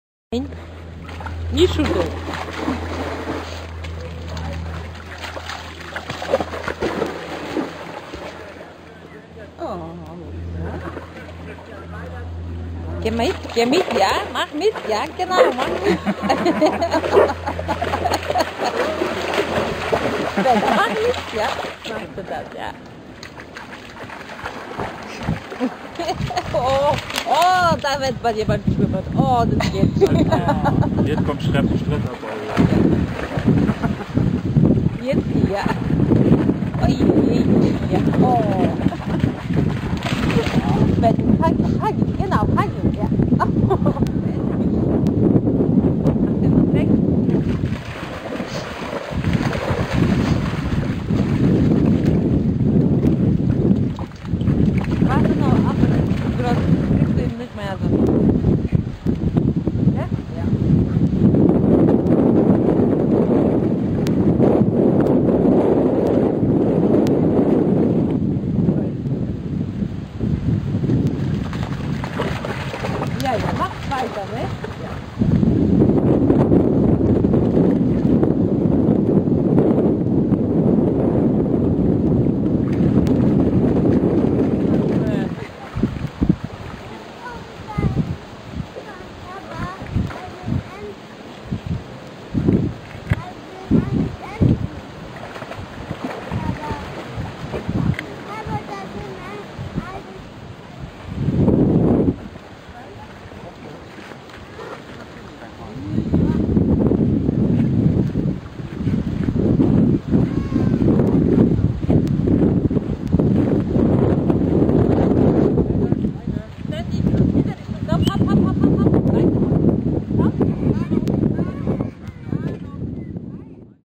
Joschi lernt schwimmen
Bisher hat Joschi auch schon sehr gerne im Wasser geplanscht und ist um keine Pfütze drum herum gelaufen. Jetzt, im Alter von drei Monaten, ist er das erste Mal richtig geschwommen: Am Tag zuvor mehr zufällig, als er im Wasser um eine 65 Kilo schwere Rottweilerhündin herum gehüpft ist. Und dann heute, nachdem ihn die großen Vorbilder kräftig dazu animiert hatten.